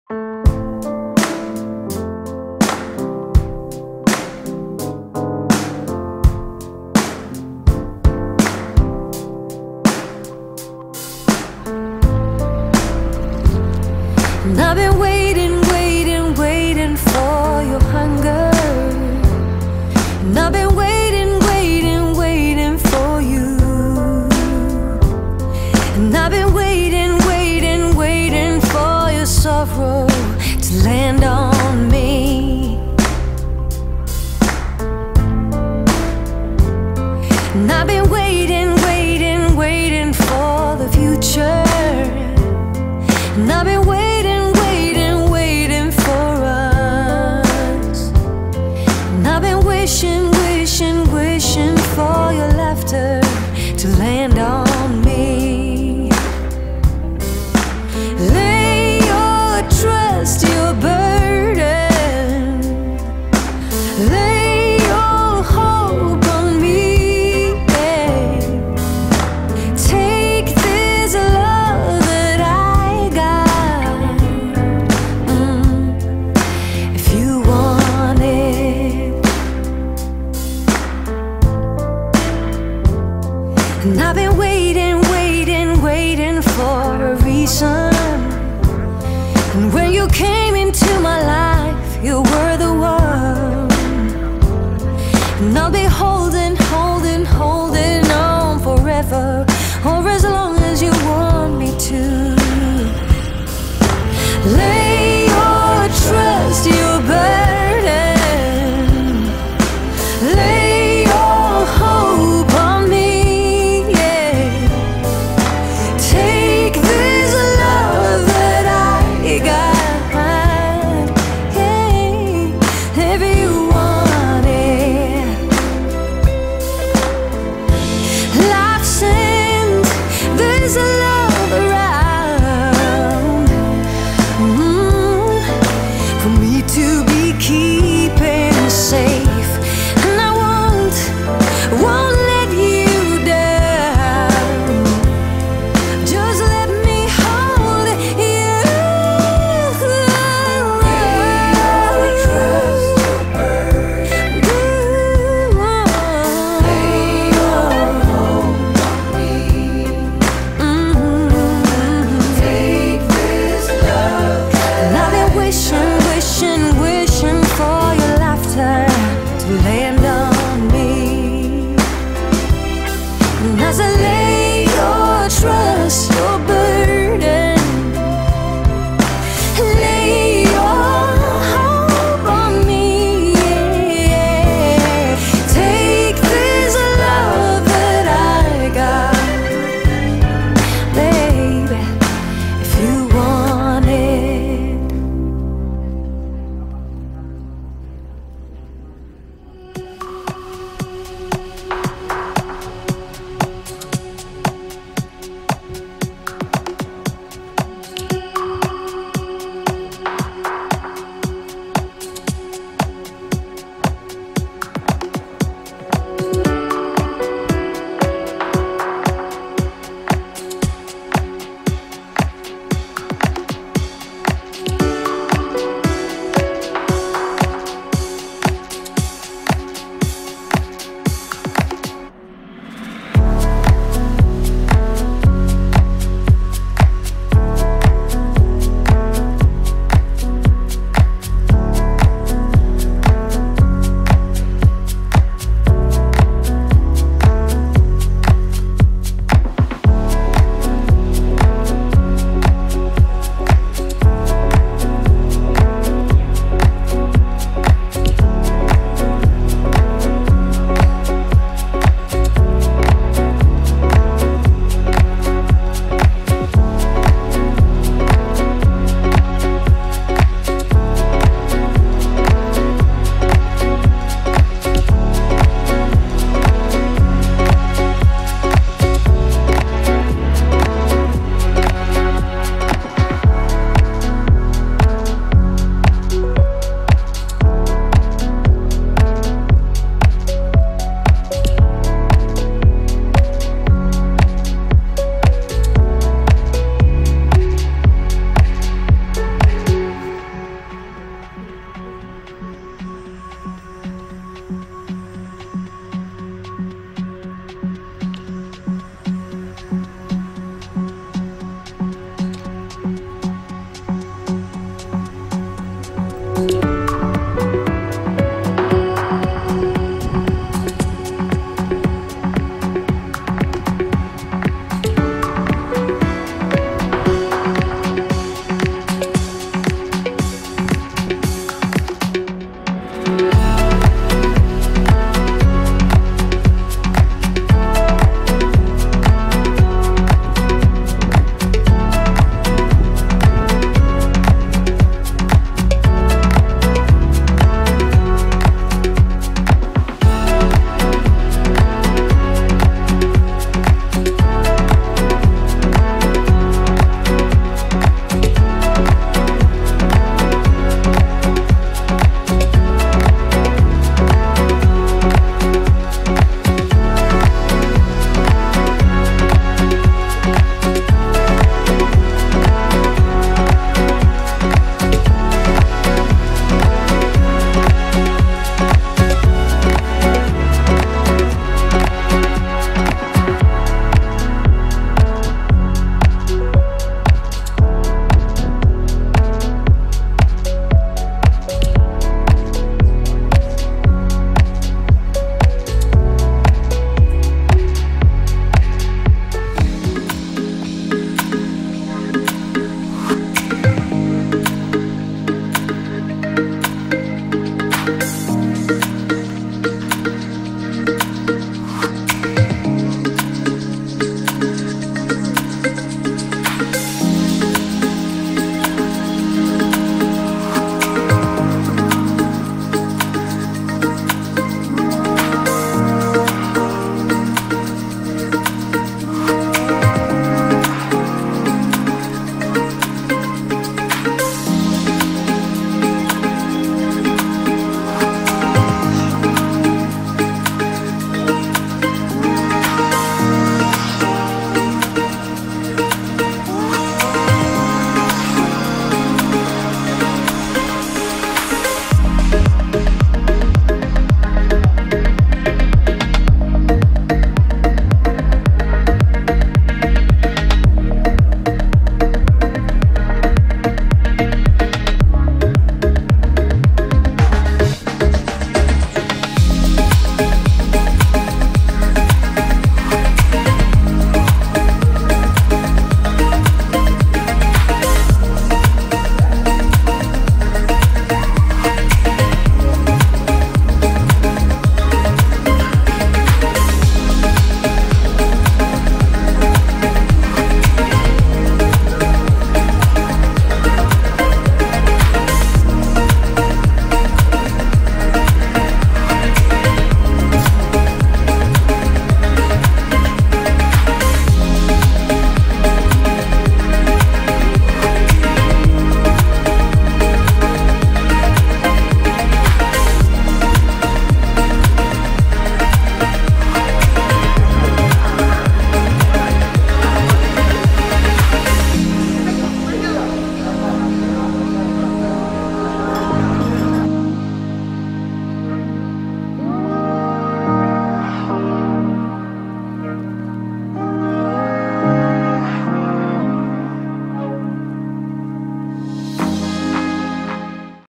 Opera House Sydney,Australia, ep 29 - travel video vlog calatorie tourism
We are in Sydney, Australia and we are visiting the Opera House
Entry is $43 for adults and $23 for children
Suntem în Sydney, Australia și vizităm Opera